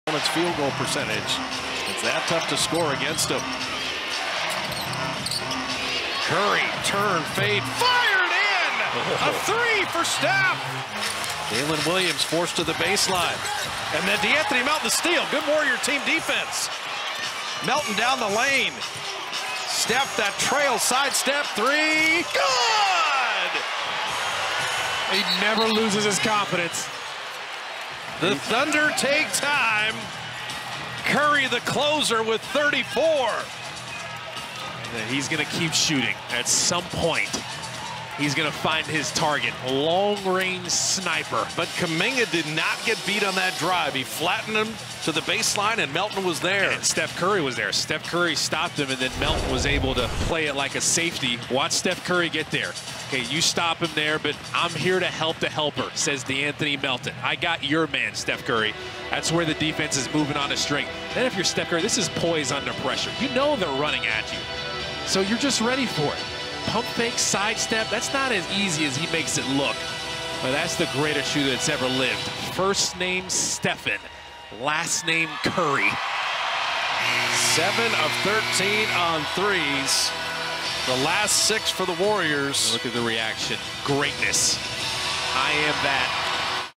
Stephen Curry Dagger 3 Puts OKC to Sleep
Steph Curry clutch scoring vs Thunder, 2024-25 NBA Season